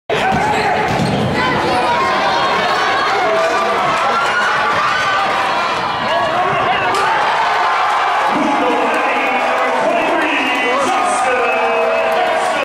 Gatson to Hairston
Melvin Gatson sets up Juston Hairston off the backboard in the AMC Quarterfinals